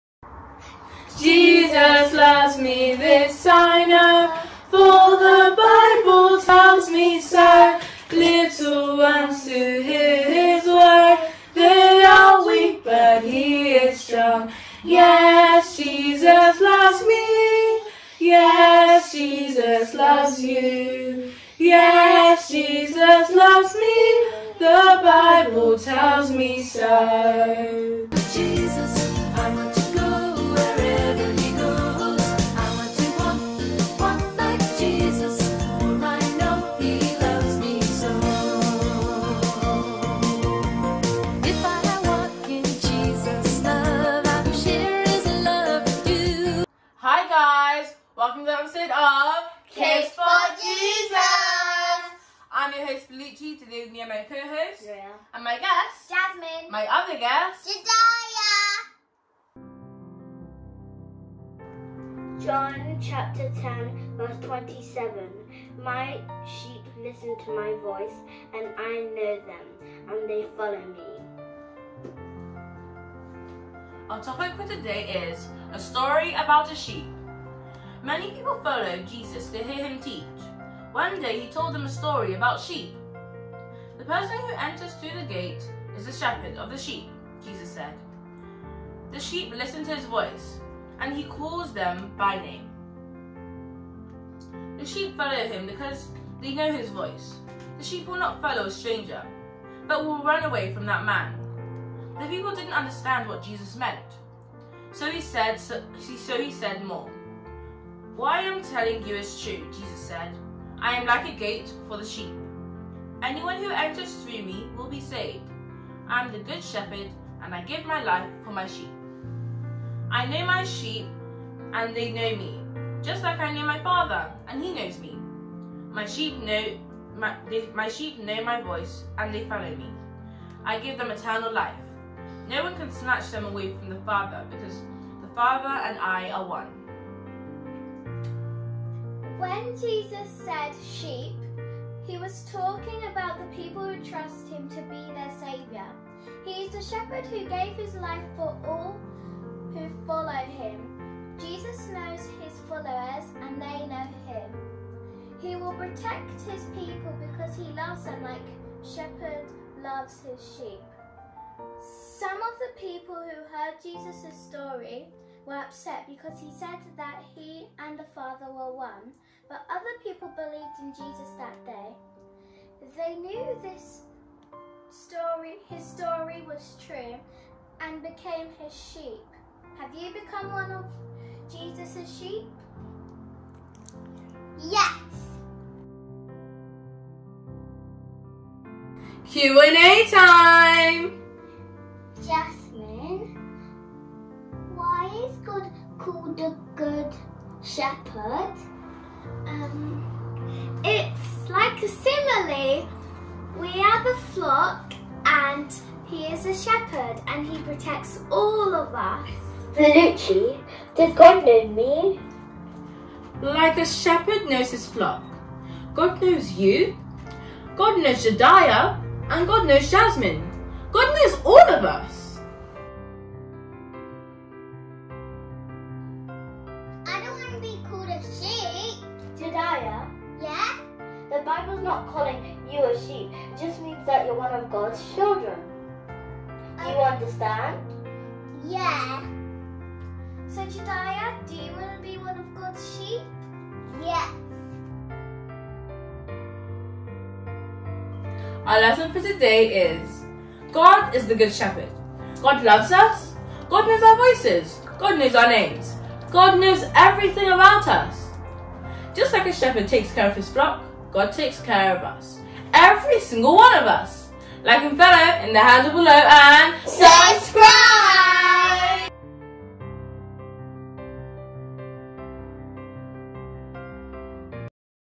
The Shepherd
This story from Our Daily Bread for Kids by Crystal Bowman & Teri McKinley teaches us about The Good Shepherd and his sheep.